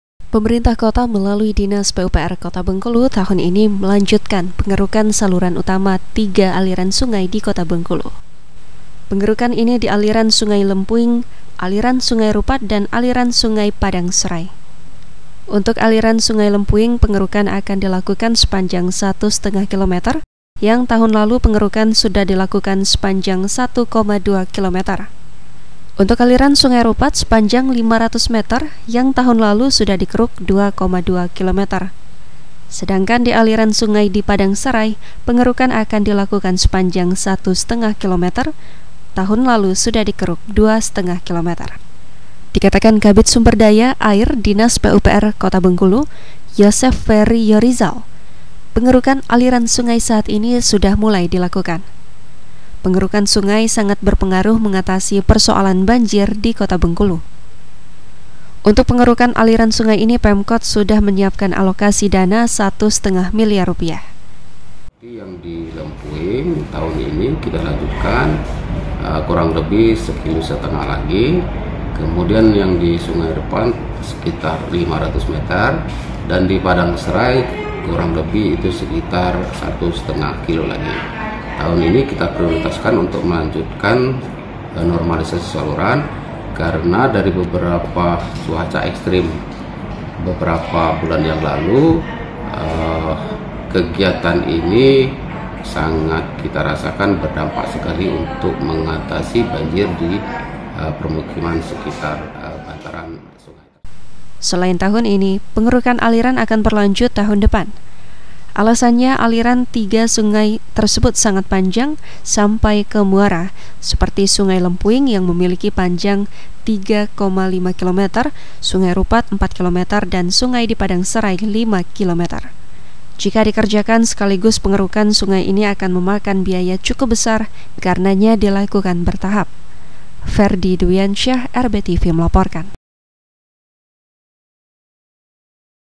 Pemkot Lanjutkan Pengerukan Tiga Aliran Sungai Utama di Kota Bengkulu
Tahun ini Pemerintah Kota Bengkulu akan melanjutkan pengerukan saluran utama tiga aliran sungai di Kota Bengkulu. Pengerukan ini di aliran sungai Lempuing, aliran sungai Rupat dan aliran sungai Padang Serai. Dengan dilakukan pengerukan, Pemkot yakin akan mengatasi persoalan banjir.